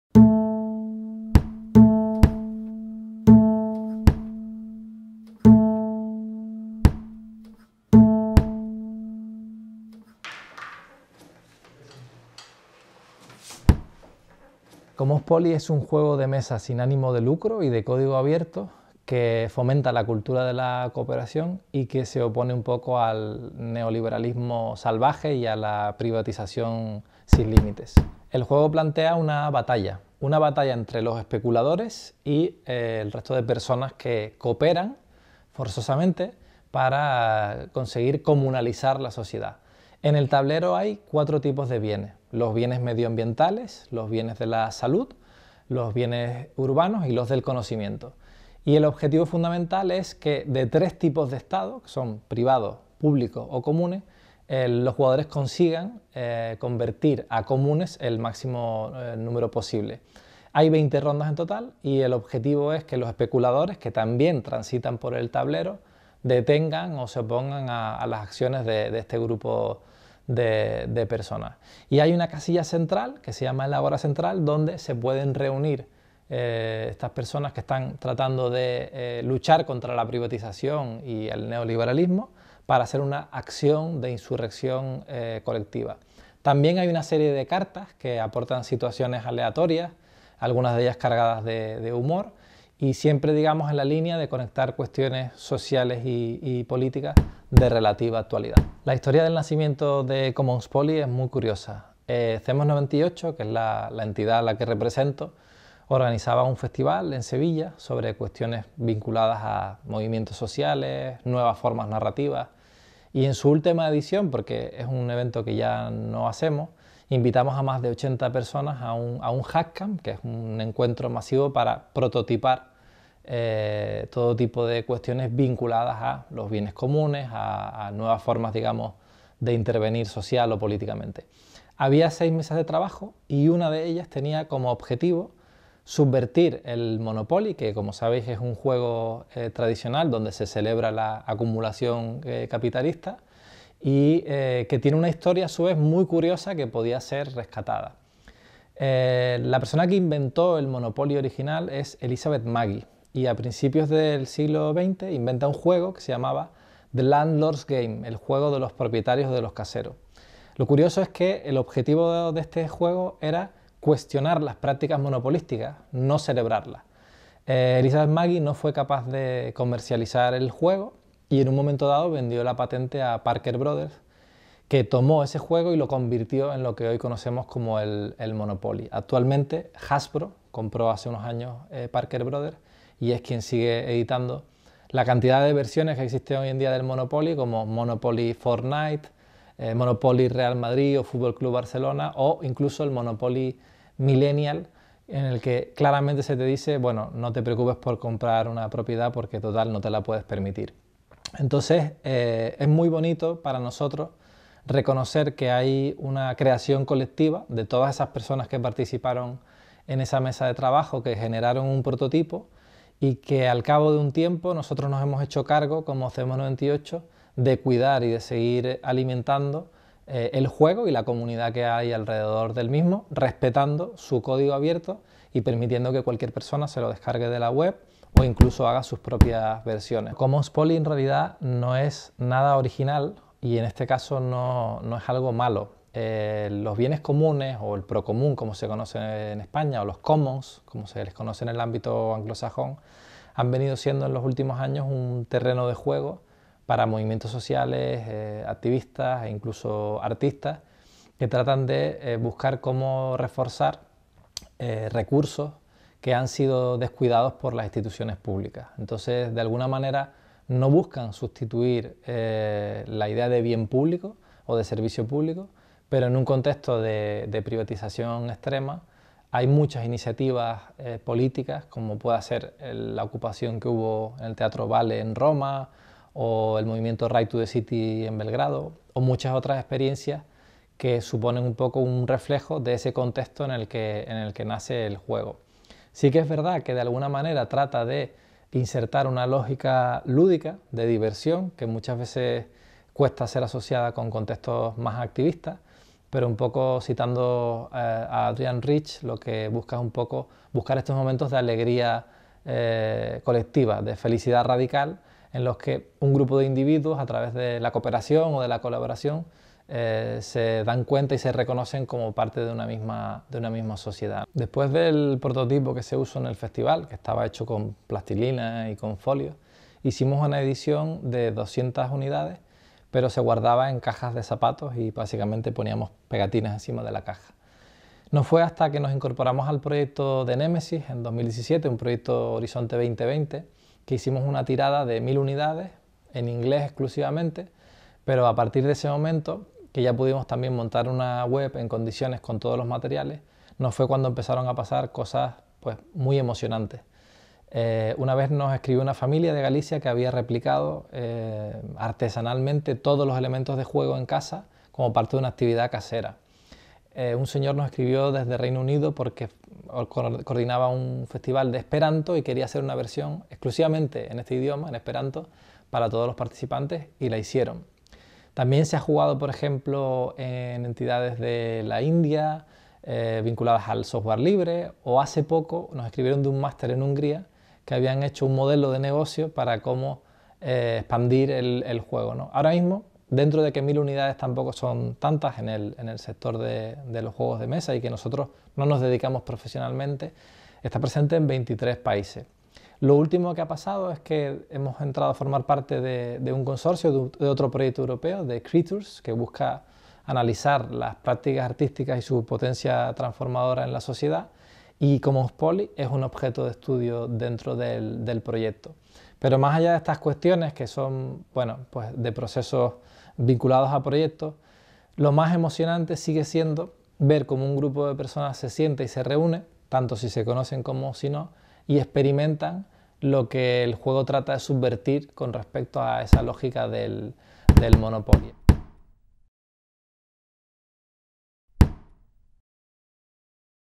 Commonspoly. Nos necesitamos. ¡Vamos a cooperar!
Presentación del juego de mesa Commonspoly para el festival internacional de juegos de mesa educativos Gaming for the Commons realizada por Felipe G. Gil.